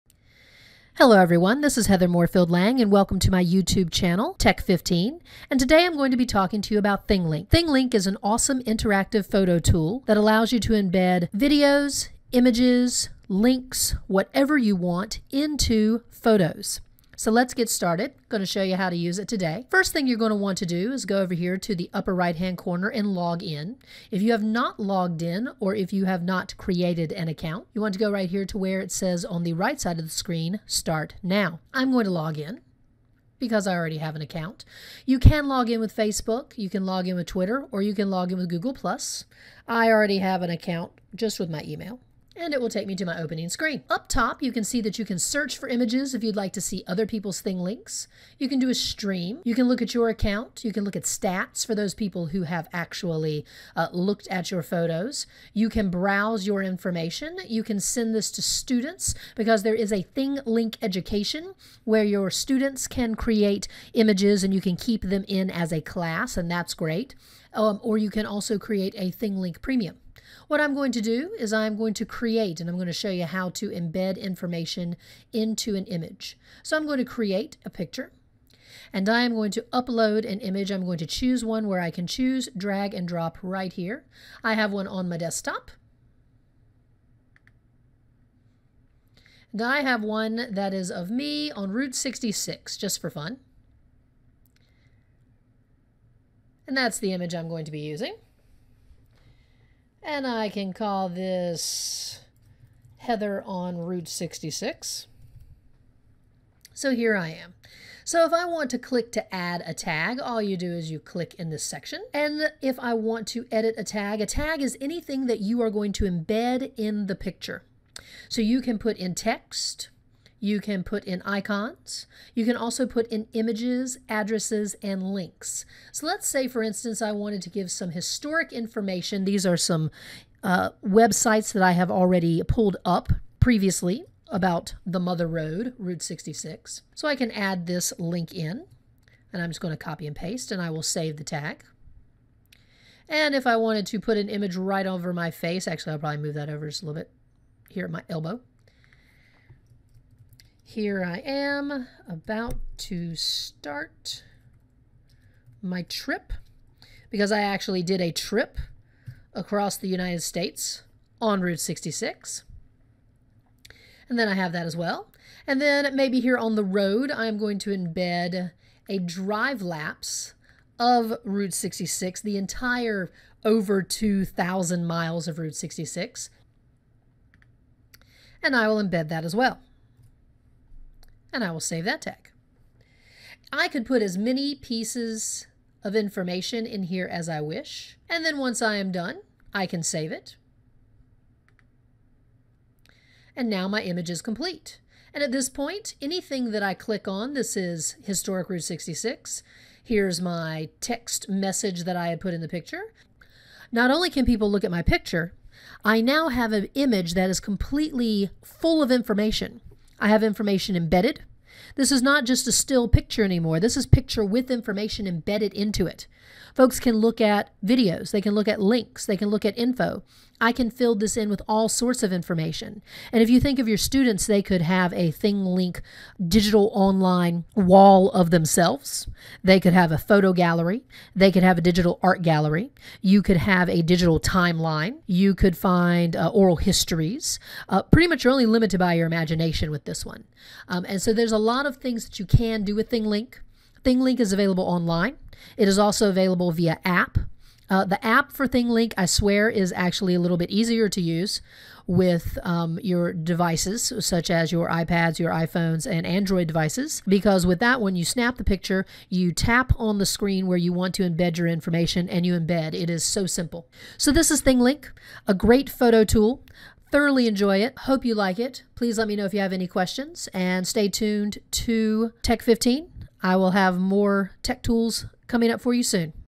Thinglink: Embedding Information into Pics
Short video on how to use the online and app tool Thinglink.